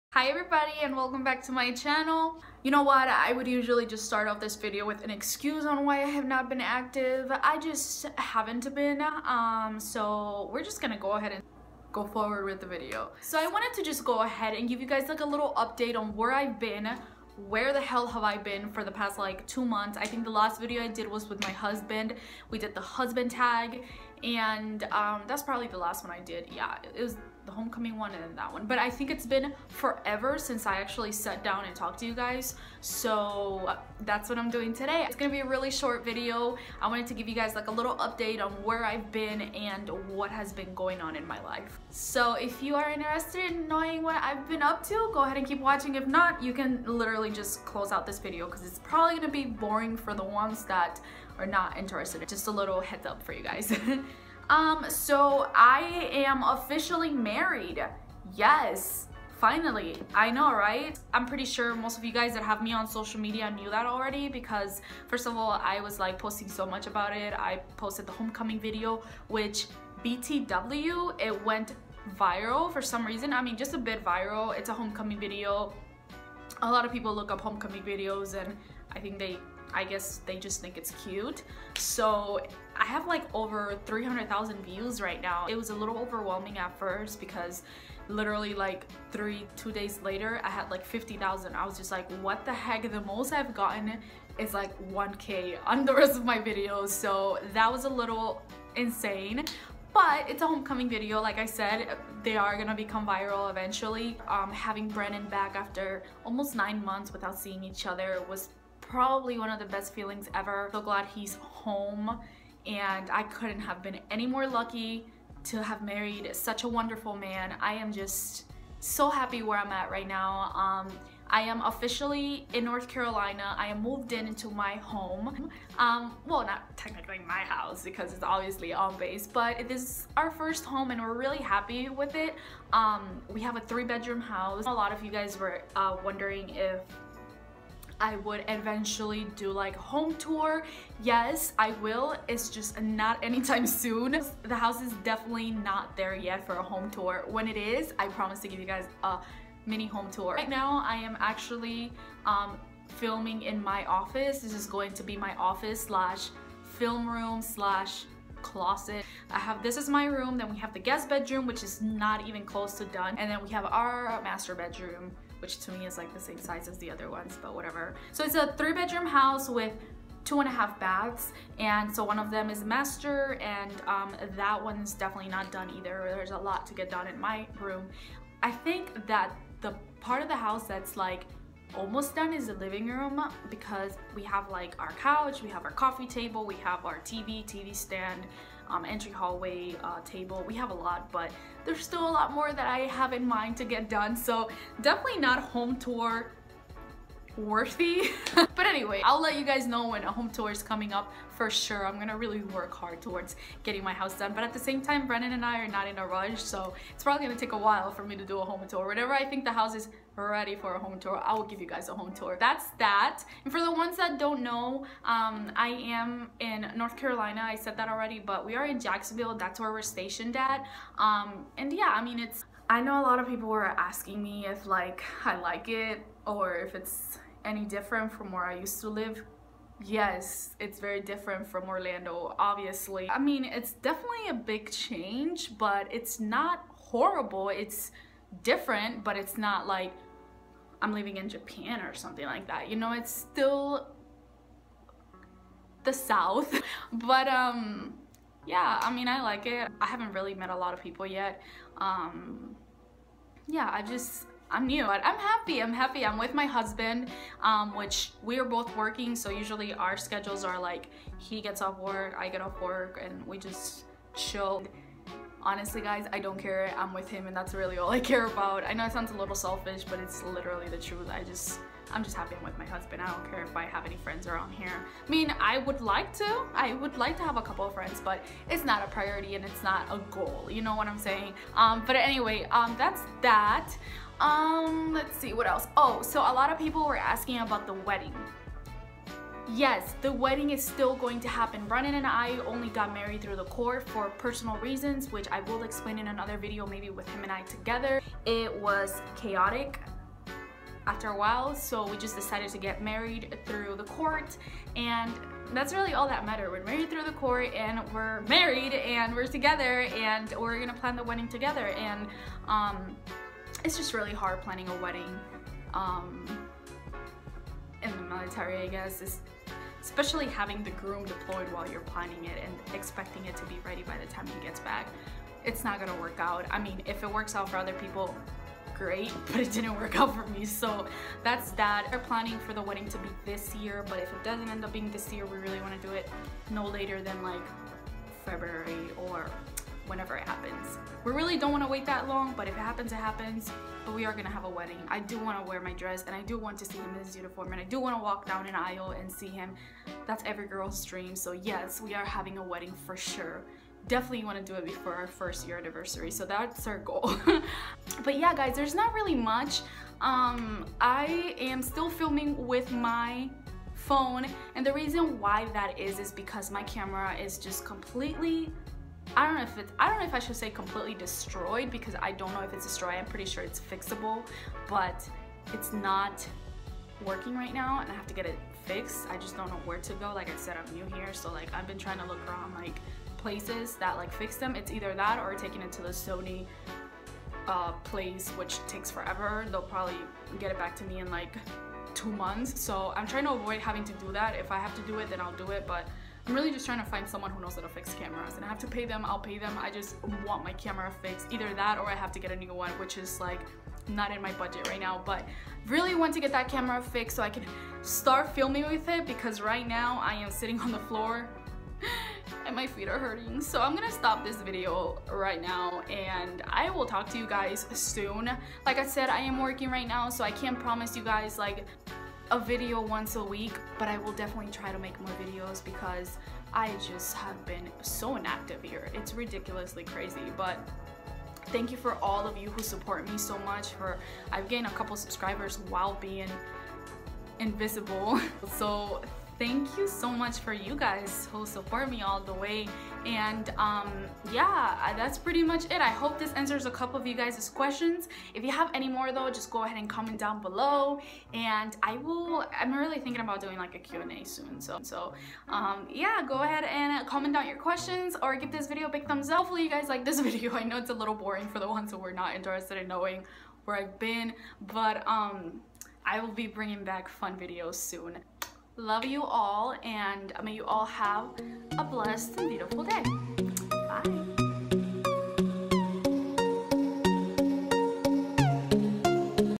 WELL HELLO! | Francis Martinez
Just a short update on where the HELL I've been lol thank you so much for patiently waiting for my videos. LOVE YA xx